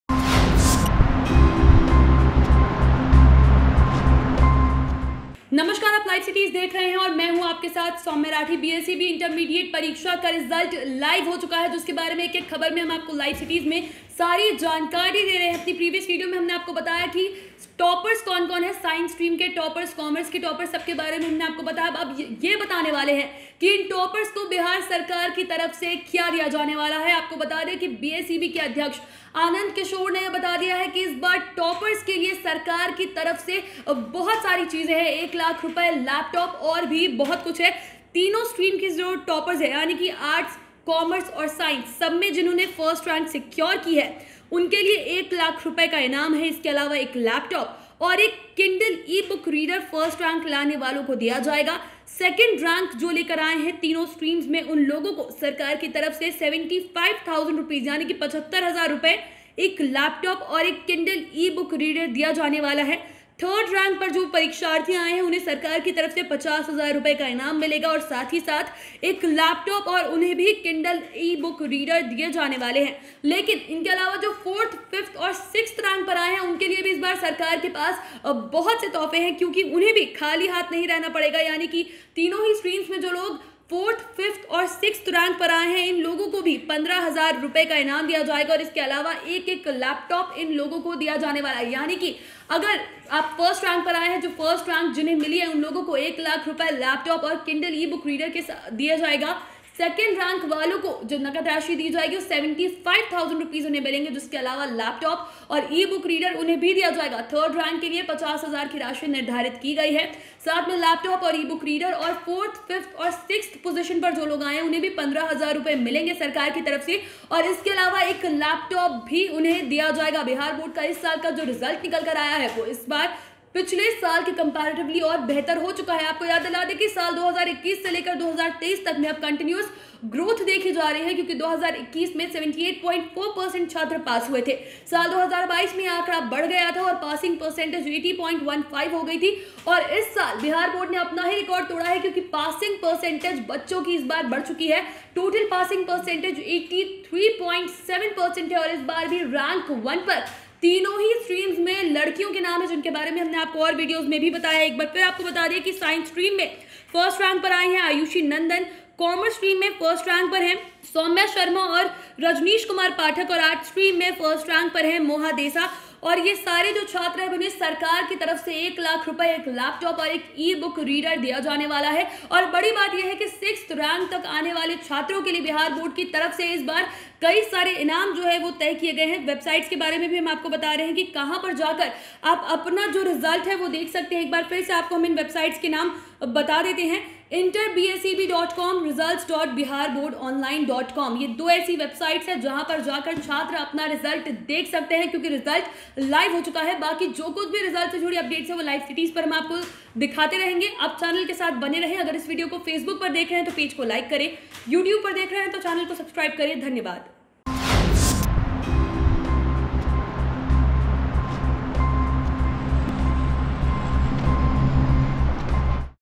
Bihar Board Inter Result 2023 : Toppers को सरकार देगी लाखों, Laptop-Tab भी, इस बार इनको भी मिलेगा..
Bihar Board Inter Result 2023 : Toppers को सरकार देगी लाखों, Laptop-Tab भी, इस बार इनको भी मिलेगा..| Bihar News | LiveCities